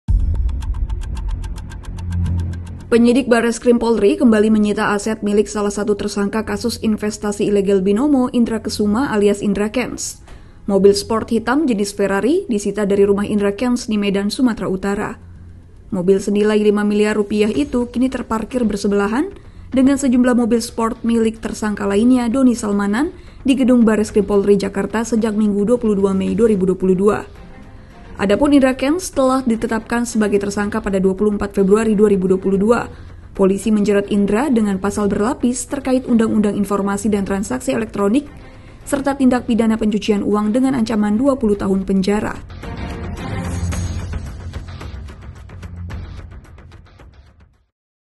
Disita Polisi, Mobil Ferari Indra Kenz Parkir di Bareskrim
Penyidik Bareskrim Polri kembali menyita aset milik salah satu tersangka kasus investasi ilegal Binomo, Indra Kesuma alias Indra Kenz. Mobil sport hitam jenis Ferari disita dari rumah Indra Kenz di Medan, Sumatera Utara.